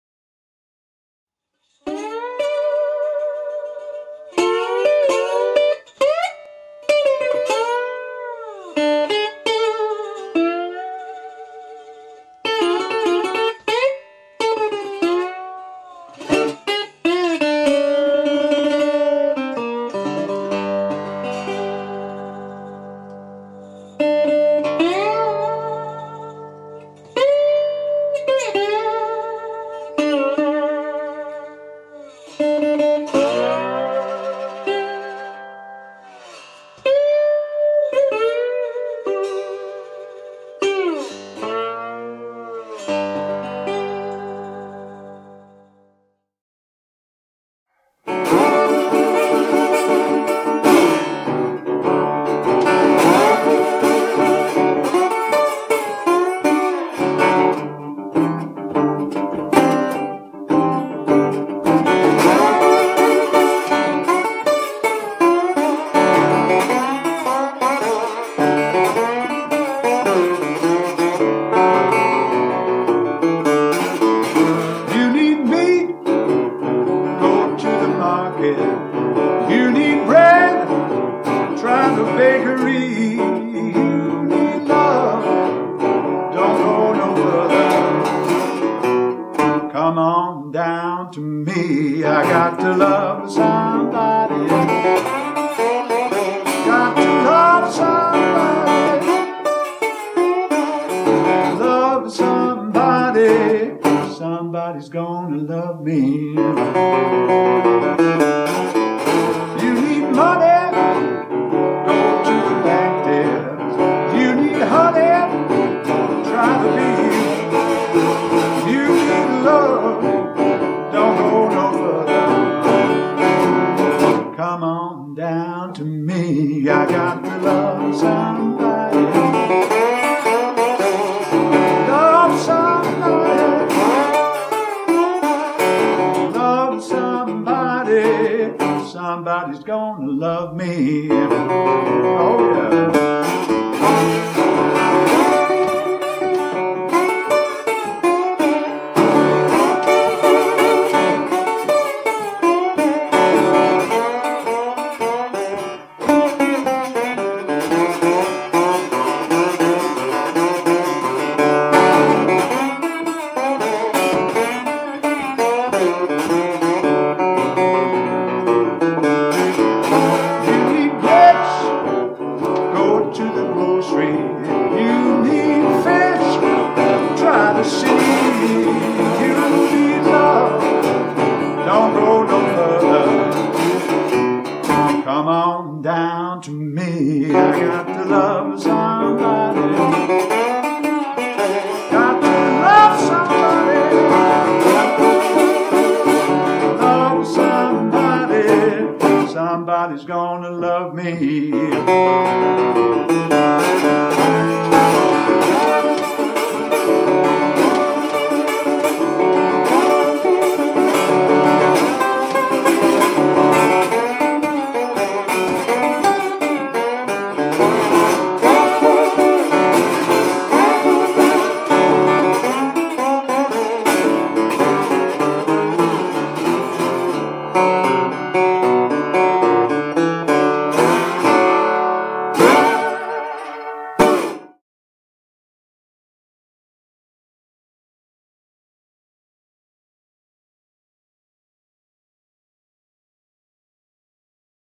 Icarus Brass Body Duolian Resonator- Antique Copper Finish
A classic 14 fret to the body duolian resonator featuring a solid brass body with 'antique' copper finish.

Thanks to Pete Harris for his fine playing, follow Pete